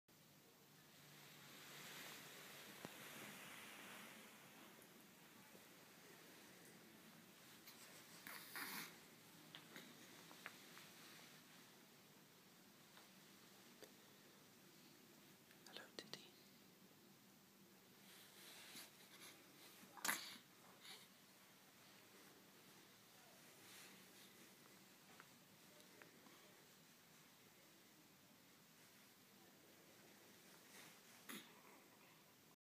Teddy one day old
Teddy Turner just hours after he was born.